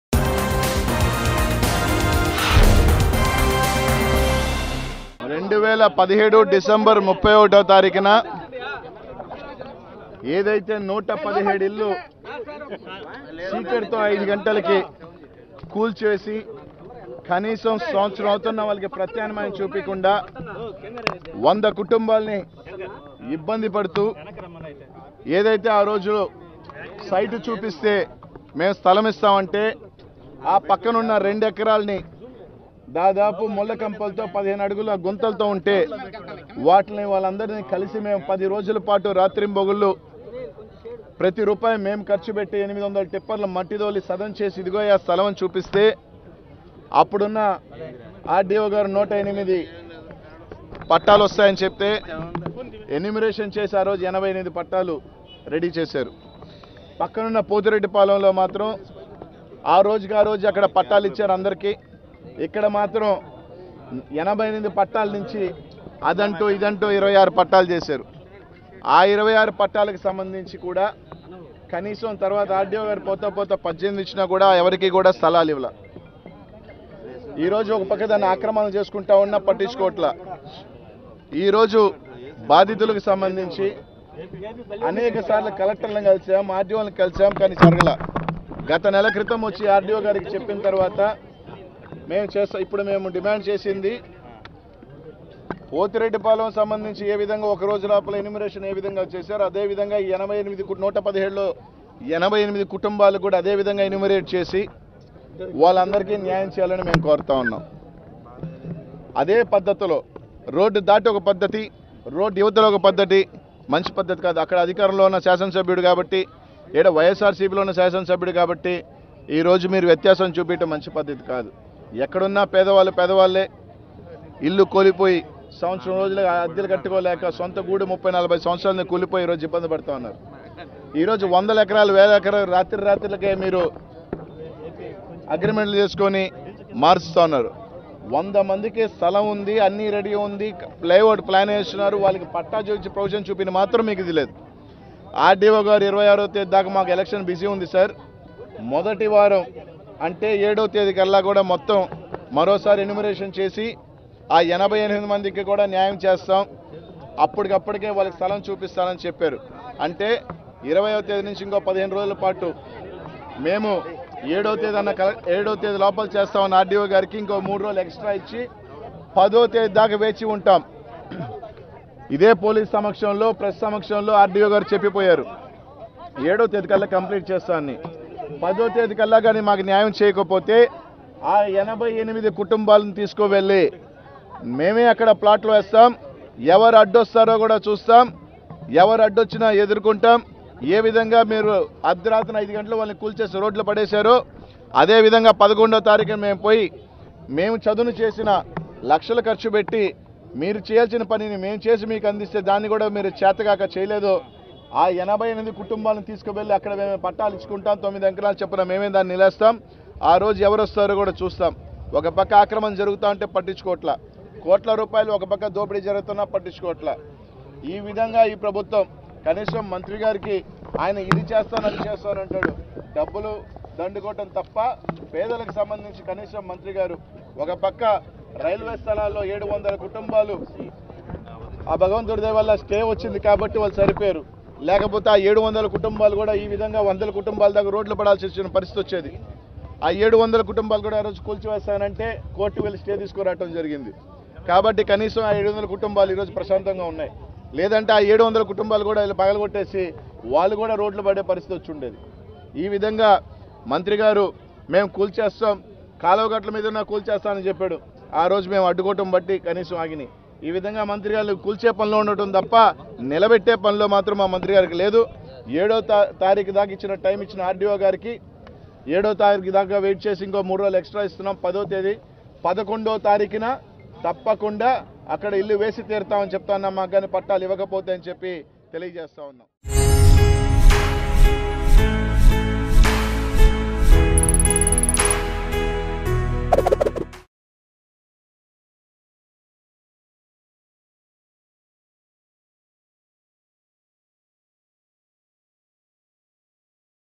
YCP MLA Anil Kumar Yadav Protest Against Poor People's House Demolition in Nellore | Mango News
YCP MLA Anil Kumar Yadav Protest Against Poor People's House Demolition in Nellore. He slams on them for demolishing houses without giving documents. He also questioned officials over their charging poor people and said he will not move from the place until justice is done to the poor people.